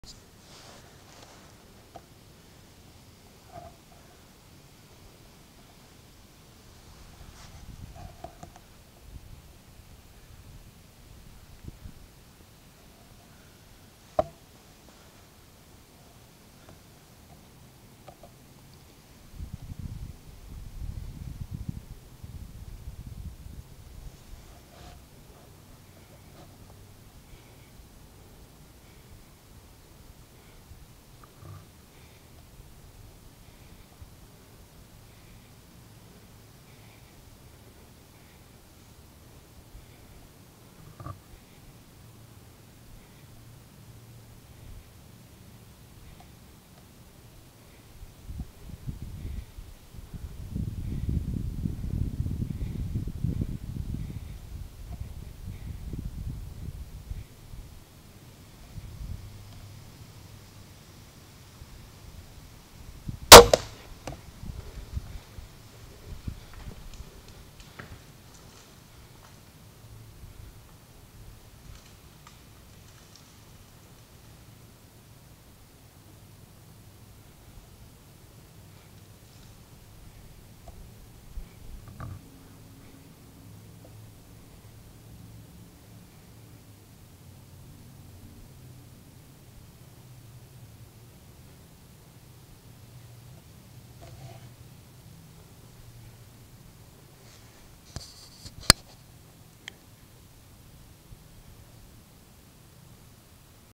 Ravin R10 and buck decoy in the woods.. 10 yards shot.
10 yard shot using a buck decoy